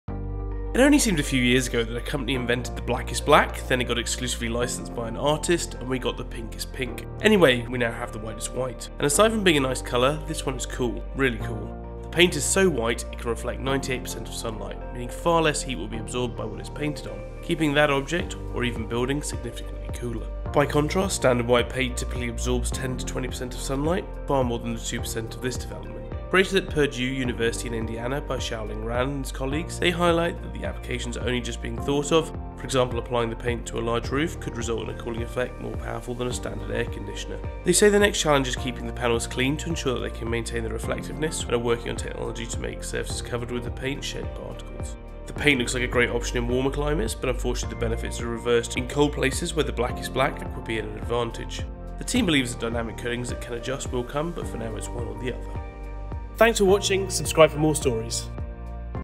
Whitest White Paint Can Cool Your Home
Xiulin Ruan & Purdue University have devised the whitest white paint ever created, and with its incredible reflective properties it could be applied to structures and roofs to keep them cool.

The paint is so white it can reflect 98% of sunlight, meaning far less heat will be absorbed by what it is painted on, keeping that object, or even building significant cooler.